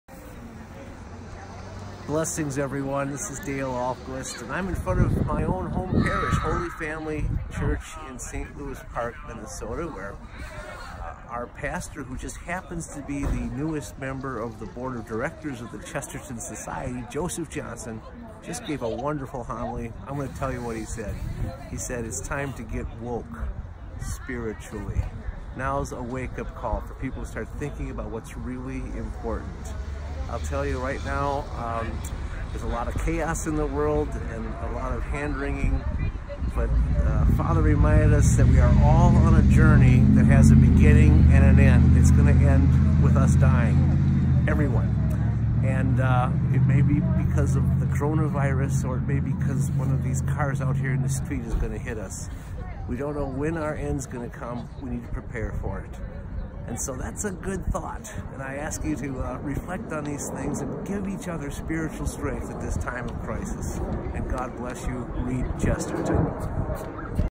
Get Woke: Dale Explains. Day 2
Dale Ahlquist shares encouragement on this daily installment of our virtual pilgrimage this Lent. Sunday, March 15, 2020.